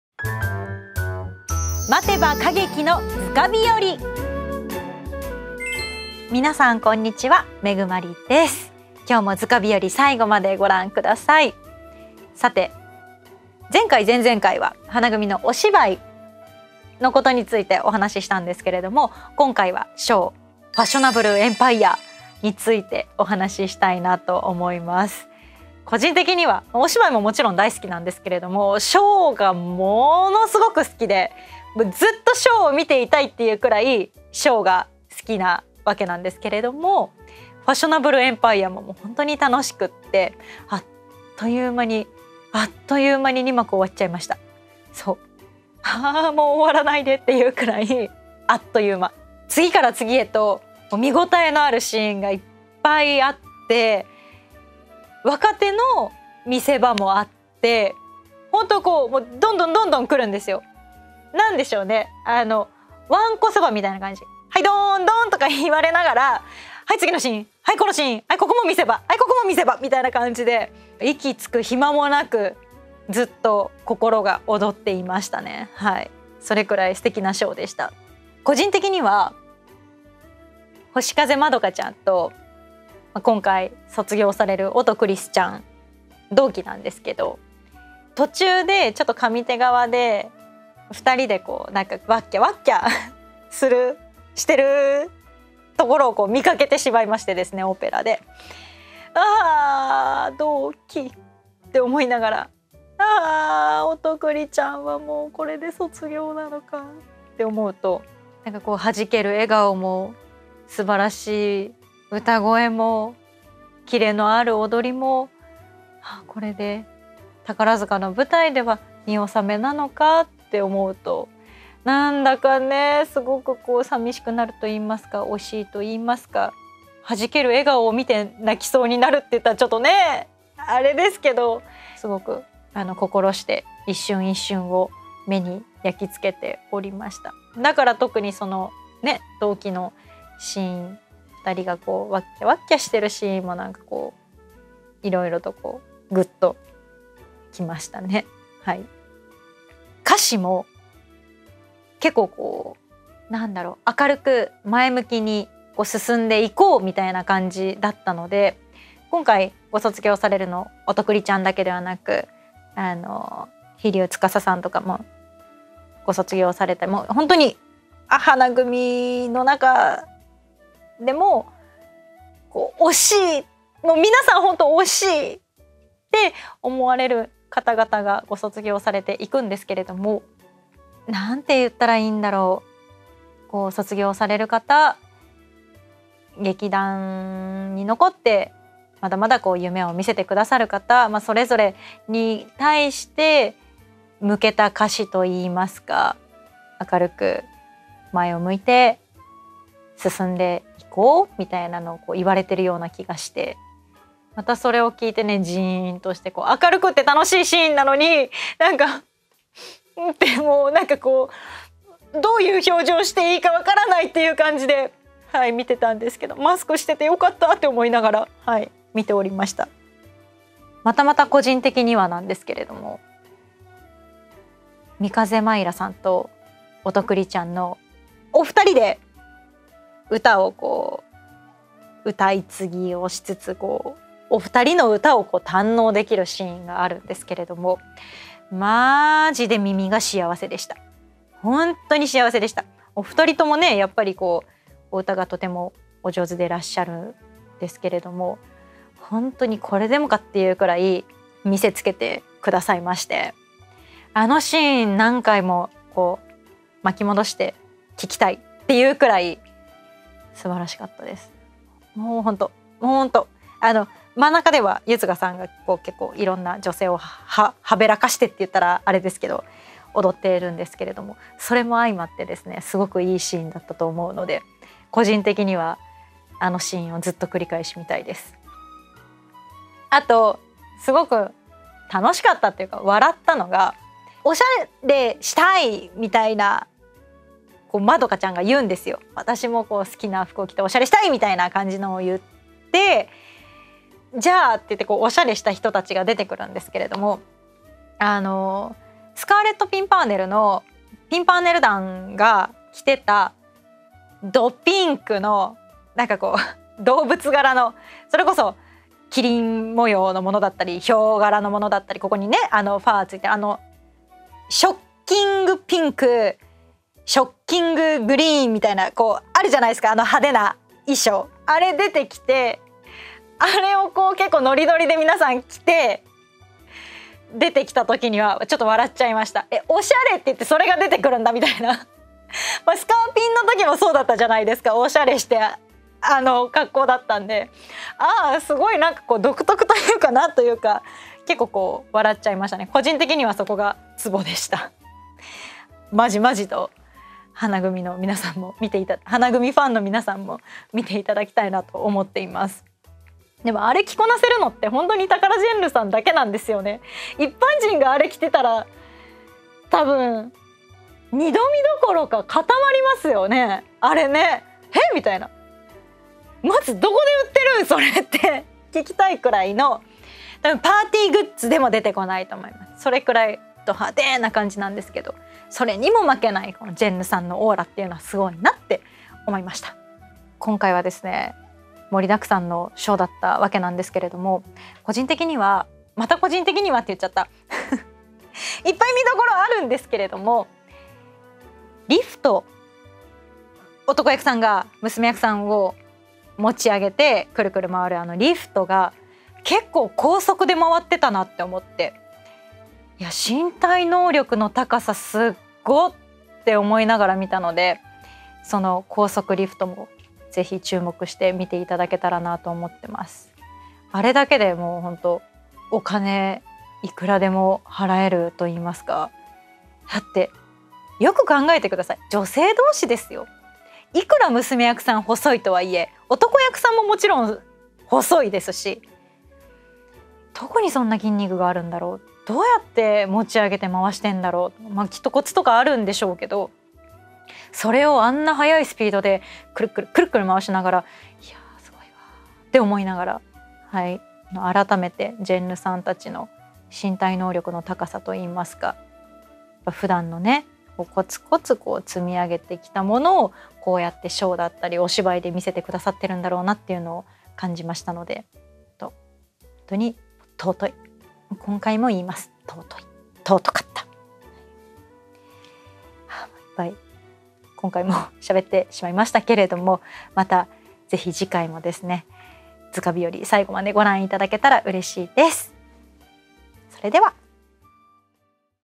花組Fashionable Empireを振り返る編【岡田愛マリーの待てば歌劇のヅカ日和】
今回のショーはまるでわんこそば？！
見どころたくさん！息つく暇もなく見せ場がどんどんやってくる…！
美風舞良さん、音くり寿さんの歌声が堪能でき
星風まどかさんと音くり寿さんの同期萌えも堪能できる今回のショー。
なんといっても一番はおしゃれ帝国…？！
宝塚で「おしゃれ」と言って出てくる“あの”衣装の破壊力に
圧倒される岡田アナなのでした。

今回もどうぞ最後までご覧ください。